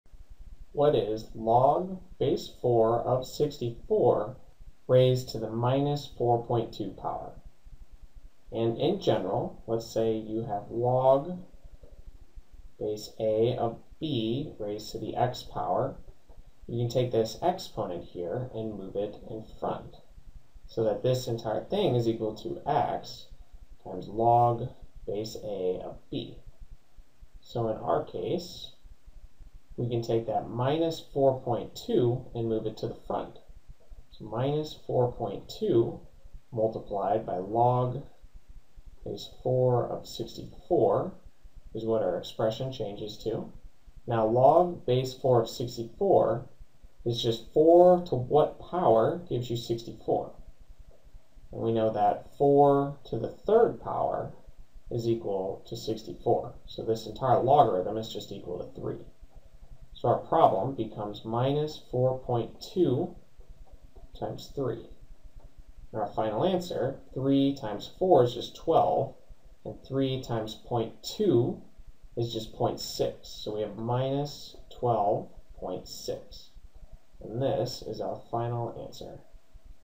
CLEP College Algebra: Logarithms 7
This is the fifty sixth problem on the CLEP college algebra practice test